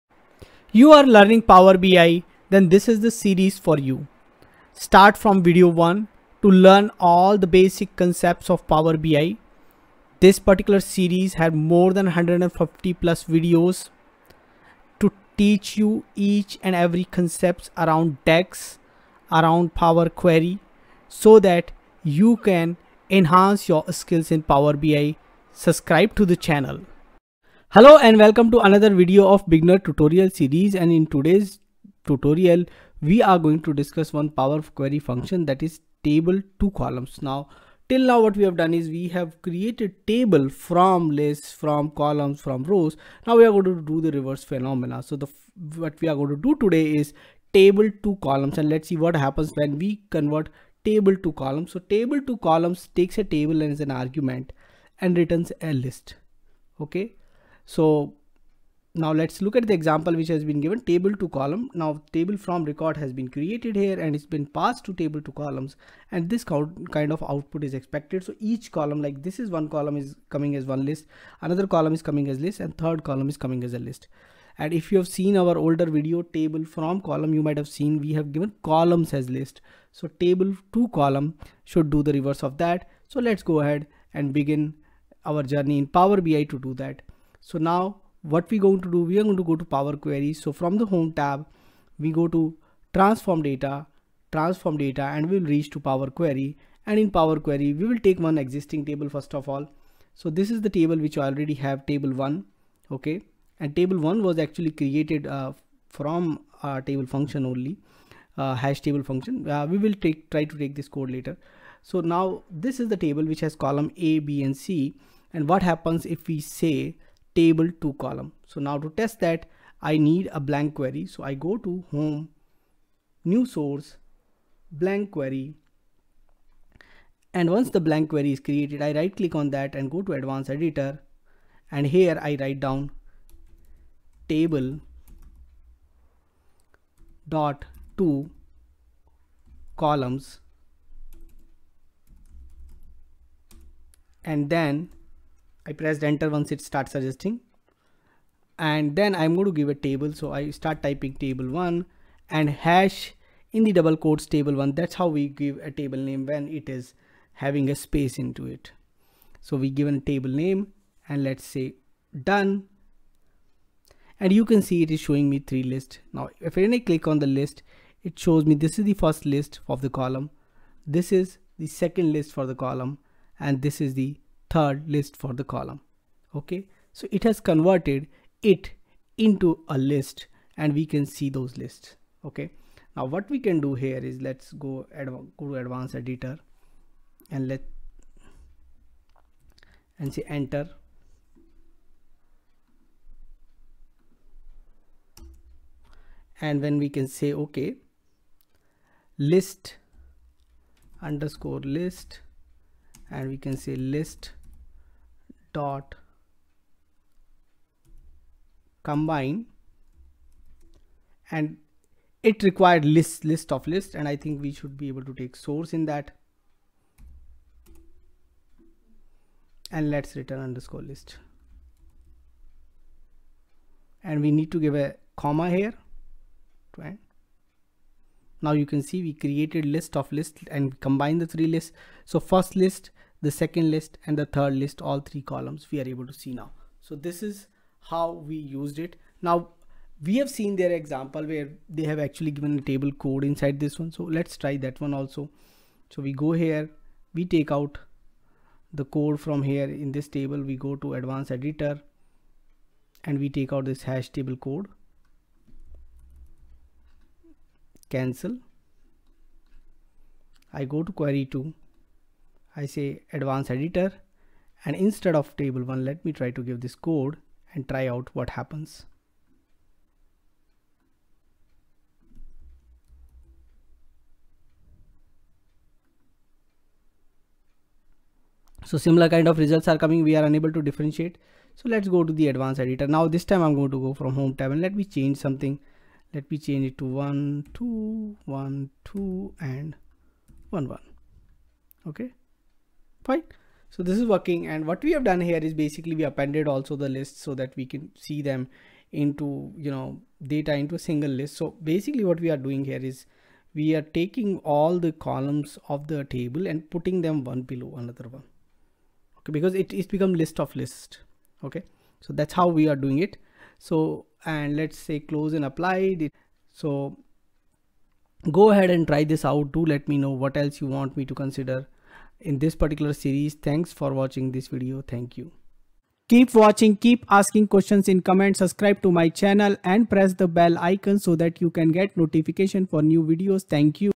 Power BI Beginners 194: Power Query Table.ToColumns
Power Query Table.ToColumns, Power BI Table.ToColumns, Power Query Table to List of columns, PL 300

--Content
00:00 Introduction
00:28  Power Query Table.ToColumns
04:00  More on Power Query Table.ToColumns

Last Video:
Power Query Table.Distinct, Power BI Table.Distinct, Power Query Remove Duplicates, Power BI Remove Duplicates